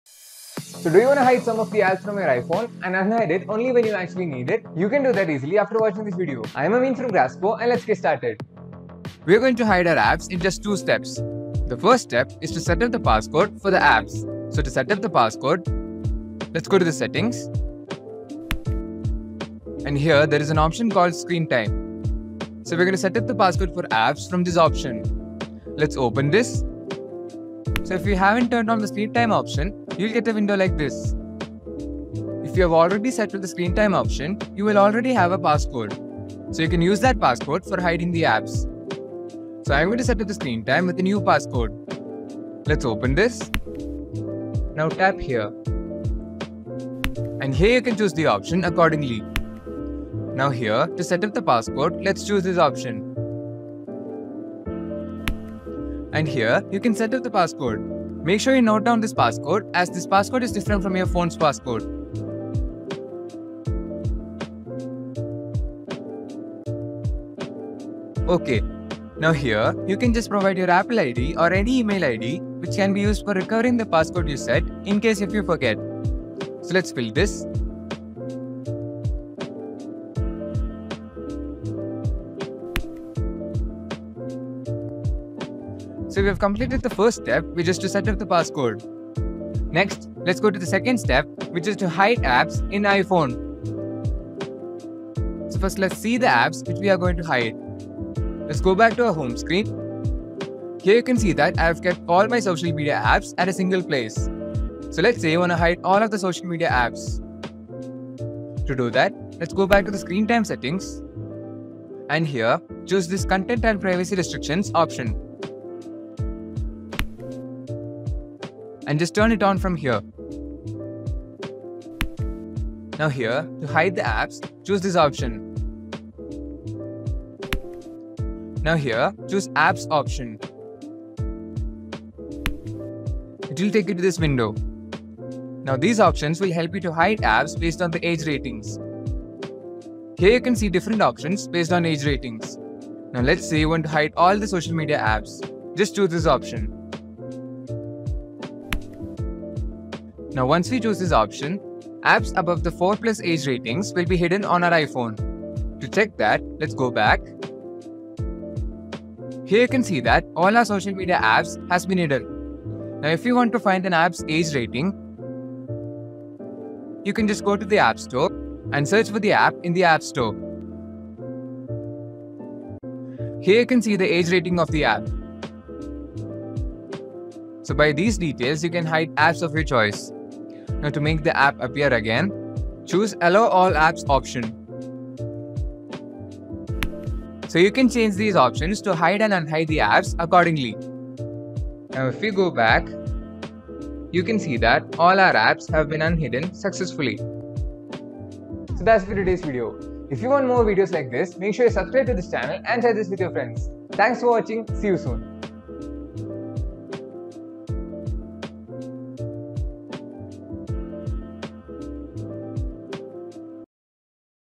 How to Hide Apps on iPhone
Learn How to Hide apps in iphone?

So, do you wanna  hide some of the apps from your iphone?
And unhide only when u actually need it?

Well this video is for you

0:00 - Intro
0:13 - Setup Passcode
1:33 - Hide Apps in iPhone
3:18 - Outro
let's say you wanna hide only  your social media apps and unhide them Whenever you want.

You can do that easily by watching this video

-woohooooooo meme effect —--

So without much further ado let's get started

We are going to hide our apps in just 2 steps

This first step is to setup the passcode for the apps

So to setup the passcode

Lets go to settings

And here there is an option called screentime

So we are going to set up the passcode for apps from this option

Let's open this

So if you haven't turned on the the screen time option

You will get a window like this

If you have already setup the set up the screen time option, you will already have a passcode, so you can use that passcode for hiding the apps

So i am going to set up the screen time with  a new passcode

Let's open this

Now here click continue, and here you can choose the option accordingly

Now, here to setup the passcode, let's choose this option

And here you can setup the passcode

Make sure you note down this passcode, as this passcode is different from your phone's passcode

Okay

Now Here you can just provide your apple id or any email ,  which can be used for recovering the passcode you set,  incase if your forget

So let's fill this ,

So we have completed the first step which is to setup the passcode

Next let's go to the second step which is to hide apps in iphone

So first lets see the apps which we are going to hide

Let's go back to our home screen

Here you can see that i have kept my all social media apps at a single place

So let's say you wanna hide all these social media  apps

To do that let's go back to the screen time settings

and  here chose this  ‘content & privacy restrictions’ option

And just turn on it from here

Here it will ask you to enter the passcode which we created in the previous step

Let's enter the passcode here

Now, here to hide the apps, choose this option

Now here choose ‘apps’ option

It will take you to this window,

Now these options will help you to hide apps based on the age ratings

Here you can see different options based on age ratings

Now let's say you want to hide all the social media apps

Just choose this option

Now once we choose this option apps above the 4+ age ratings will be hidden on our iphone

To check that let's go back

Now if you want to find a app’s age rating you can just go to app store

And search for the app in the app store

Here you can see the age rating of the app

So by these detail you can hide apps of your choice

Now to make the app appear again

choose ‘allow all apps’ option

So you can change these options to hide and unhide the apps

Accordingly

OUTRO

So we now we have Learned  the easier way to hide apps on your iphone

If you want to see more videos like this